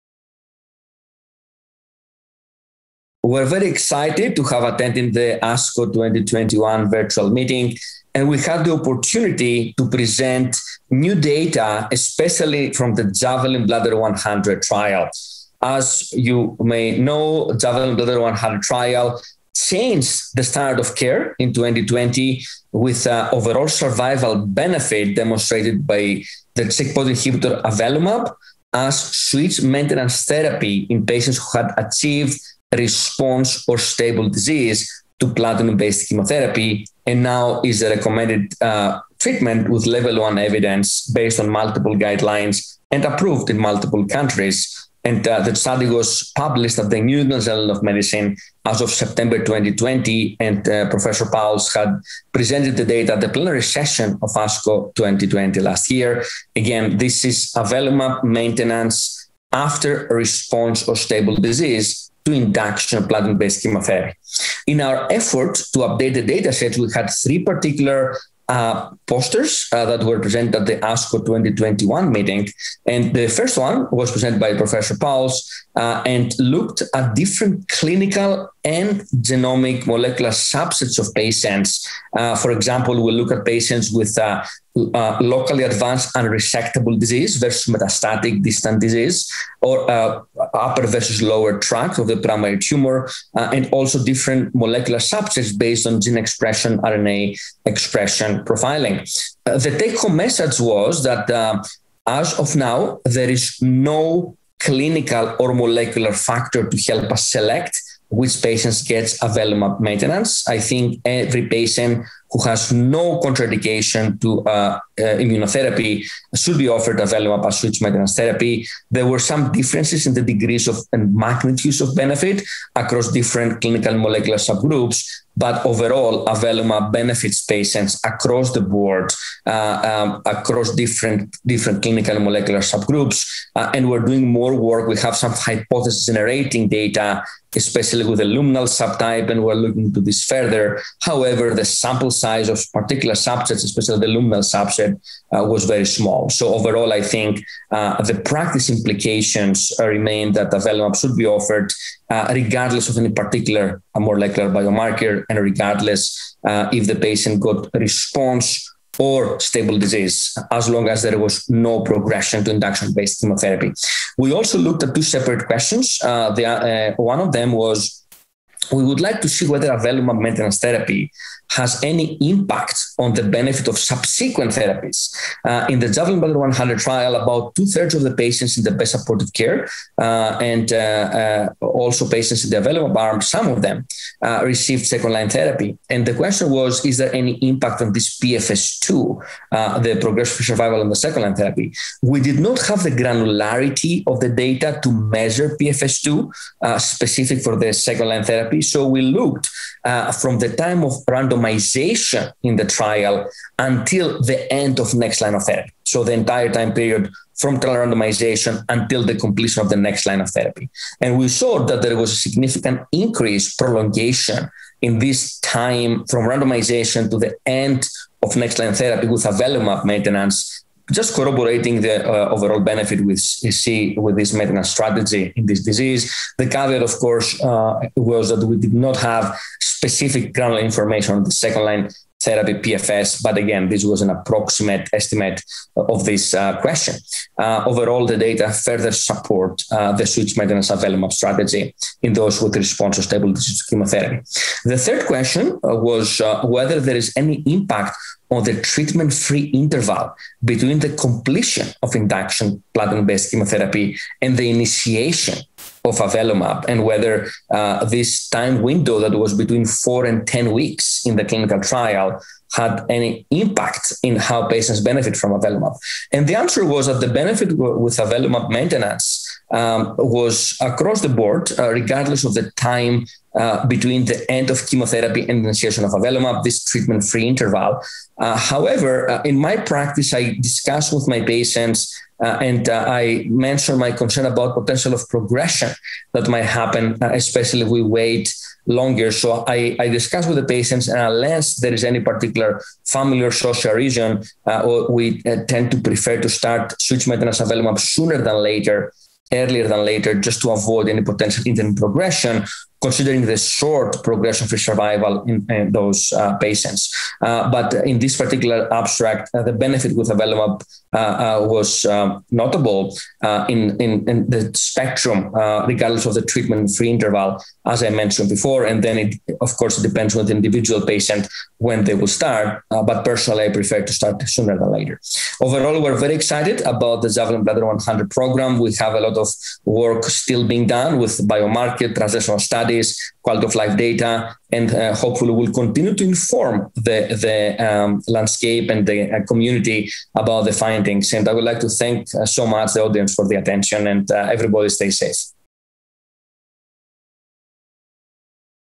JAVELIN Bladder 100 trial updates from ASCO 2021
Petros Grivas, MD, PhD, University of Washington School of Medicine & Fred Hutchinson Cancer Research Center, Seattle, WA, speaks of the change in standard of care accomplished by the JAVELIN Bladder 100 trial (NCT02603432). The study looks at the effect of the PD-L1 immune checkpoint inhibitor avelumab compared with best supportive care alone. Updates to the data set indicated several findings: there is no clinical or molecular factor which can influence the selection of patients that receive avelumab maintenance treatment. Dr. Grivas believes all patients should receive avelumab therapy regardless of any clinical or molecular biomarkers, as long as there is no contra-indication to immunotherapy. Secondly, avelumab therapy causes an increase in the time from randomization to the next line of therapy, signifying the overall survival benefit of this treatment. This interview took place at the American Society of Clinical Oncology (ASCO) 2021 Virtual Meeting.